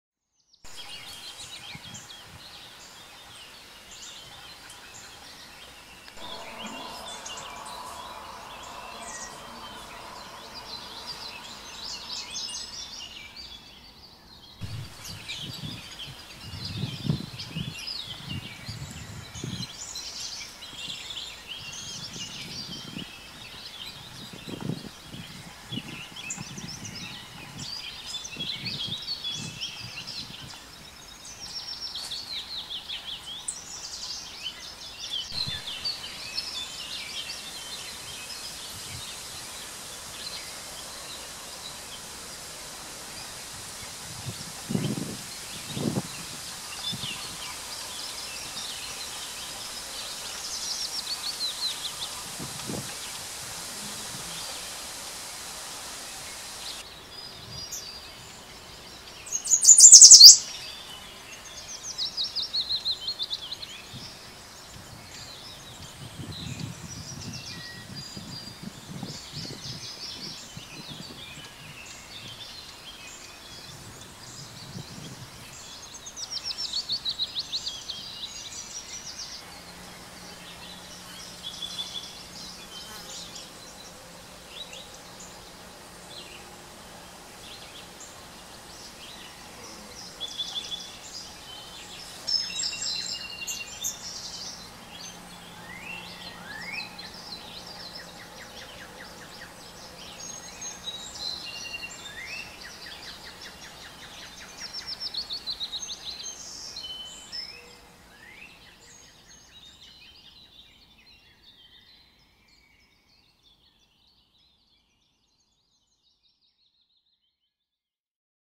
Yellow Warbler with Chicks
Yellow warbler with newly hatched  chicks nesting in an old lilac bush.